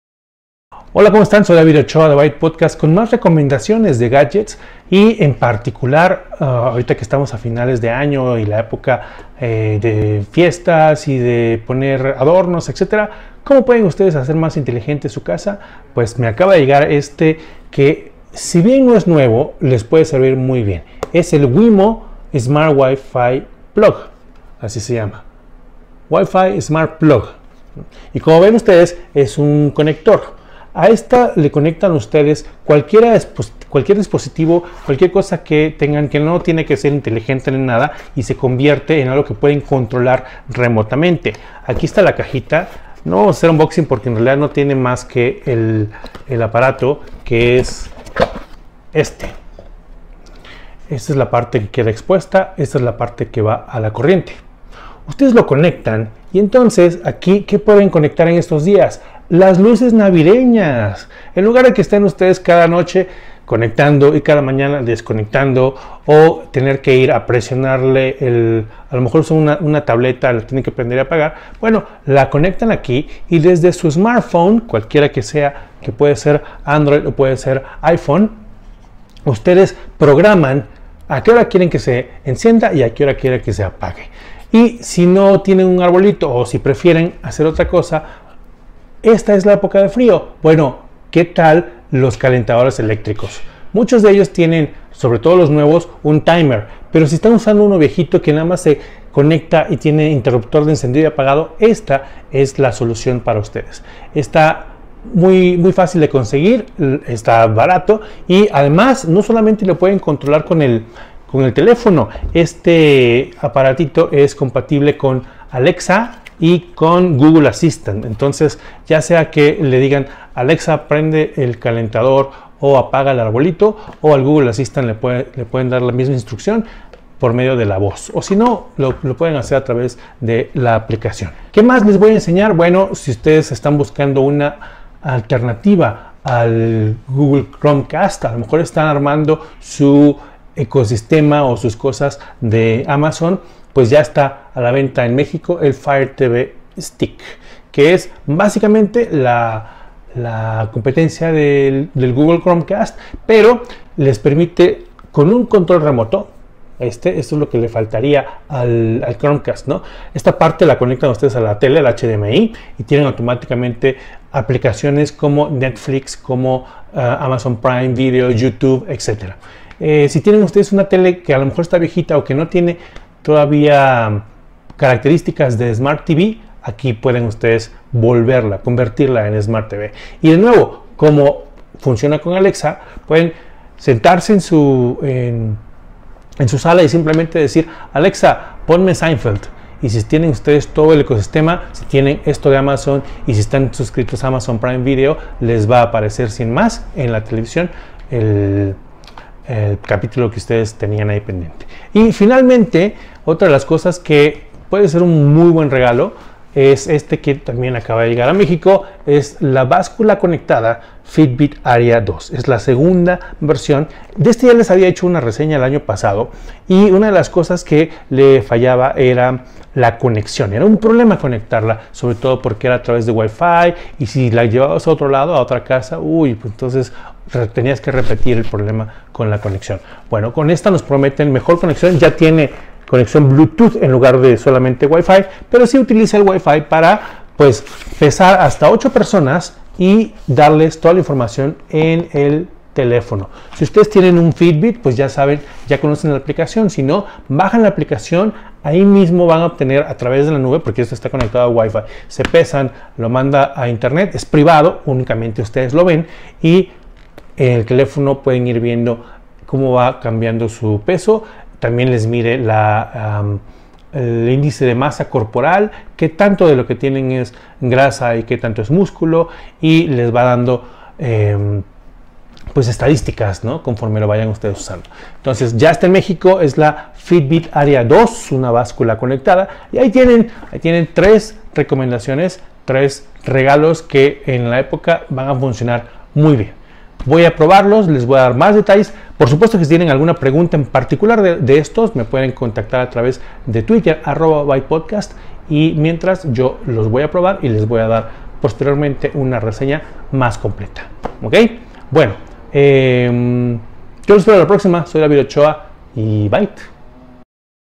Gadgets para la Casa Conectada
Les traigo 3 recomendaciones de gadgets para este fin de año, que harán que aprovechen el Wi-Fi de su casa para convertirla en Smart Home.
Wemo Wi-Fi Smart Plug
Amazon Fire TV Stick
Fitbit Aria 2